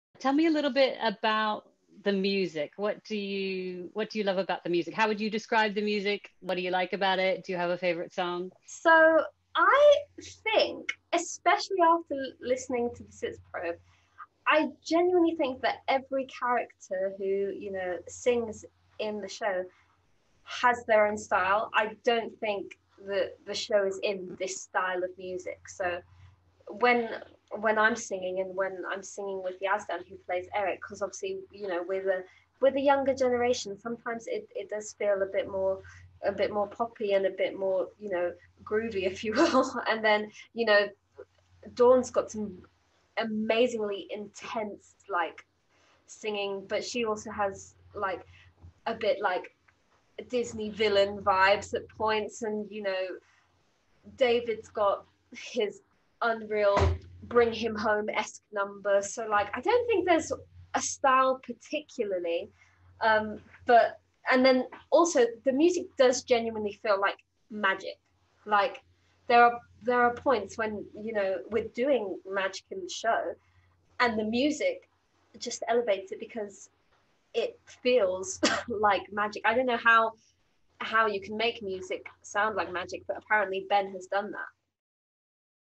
The Sorcerer's Apprentice: Mary Moore in Conversation 1 of 4
Mary Moore who plays Eva, the eponymous Apprentice, in conversation with Terri Paddock about the music in the show.